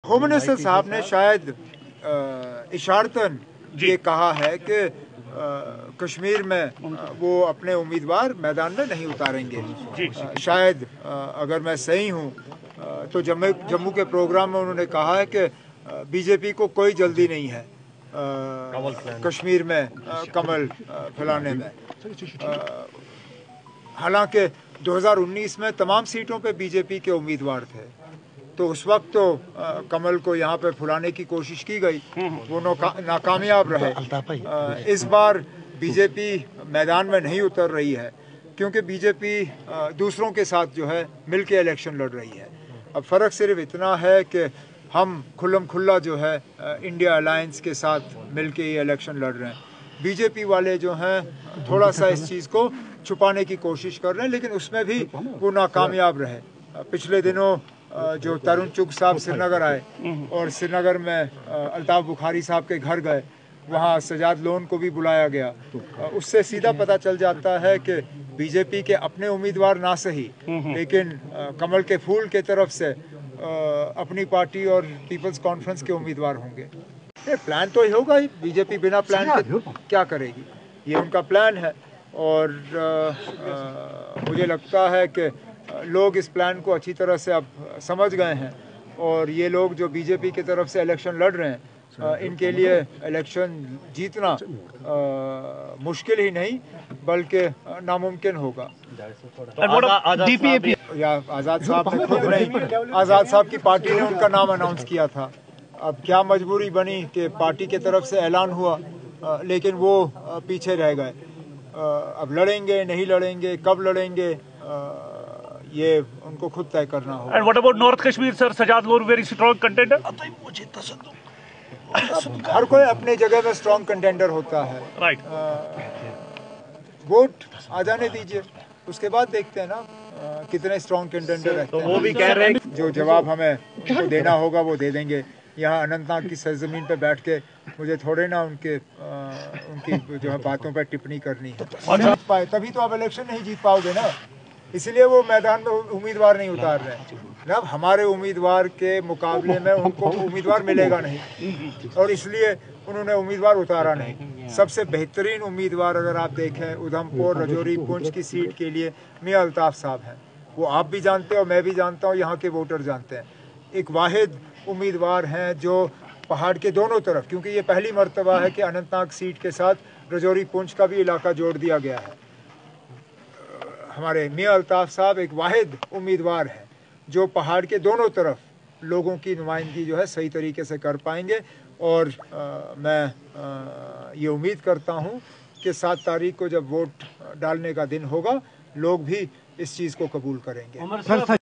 NC is overtly with INDIA alliance but A & B teams are covertly in alliance with BJP in J&K told Omar
NC is overtly with INDIA alliance but A & B teams are covertly in alliance with BJP in J&K told Omar Abdullaha in Larnoo Kokernag Anantnag.

Report : Mir Zeeshan